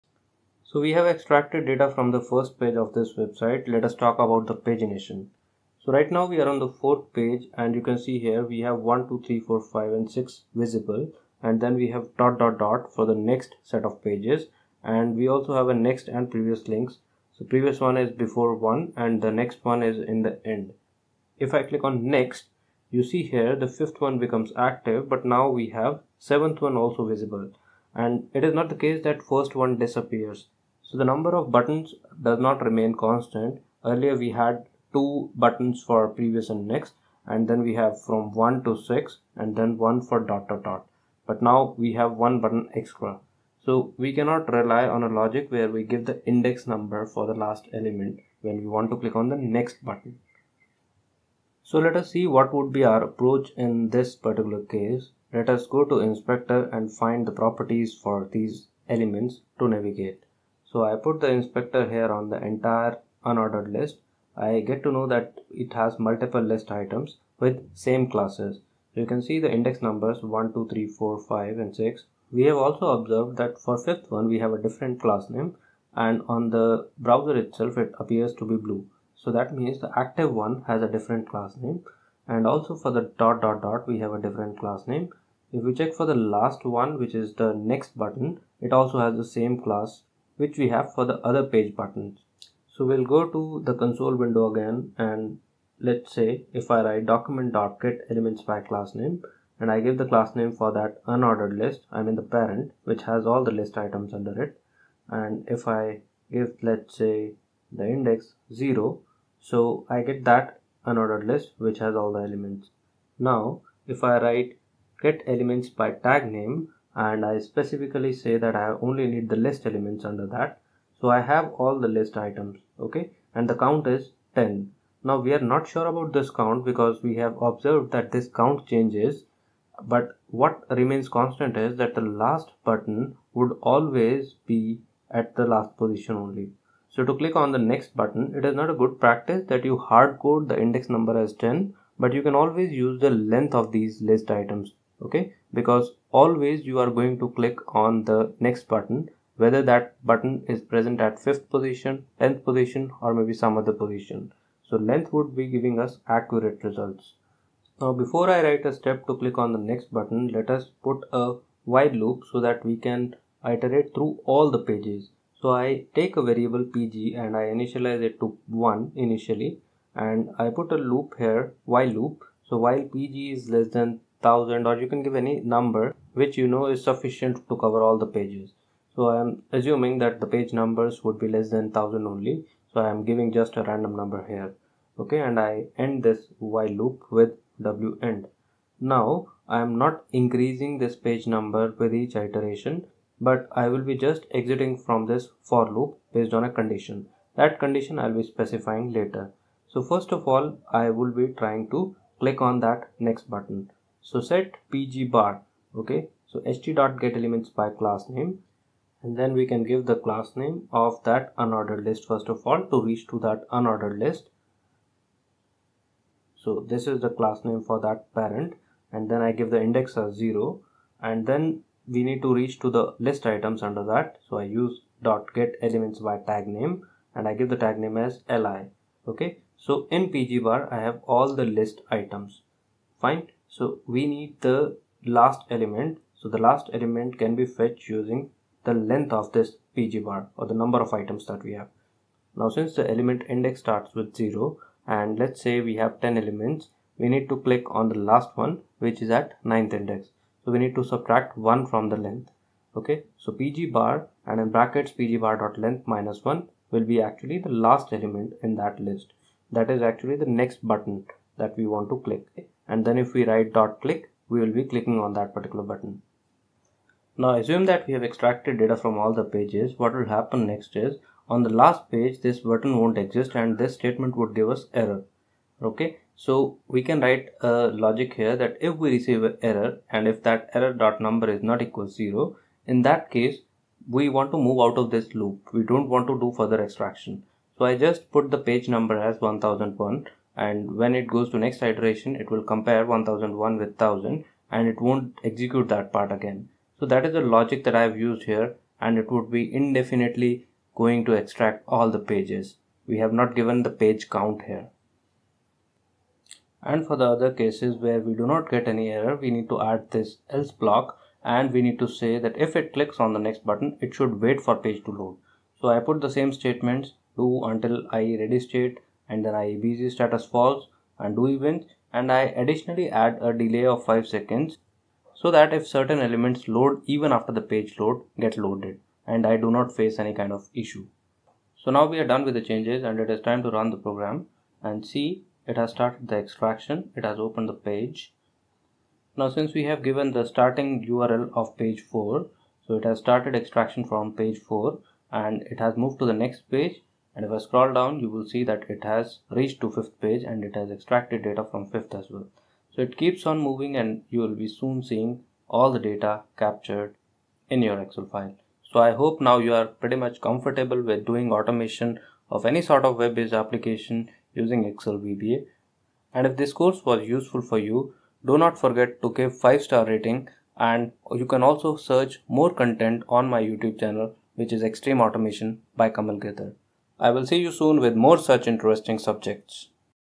Web Scraping # 18 | Pagination | Excel VBA
Many websites have pagination support (multiple pages) either by arrow buttons, page numbers , tabs or on scroll event. Let's see how to handle data extraction from multiple pages.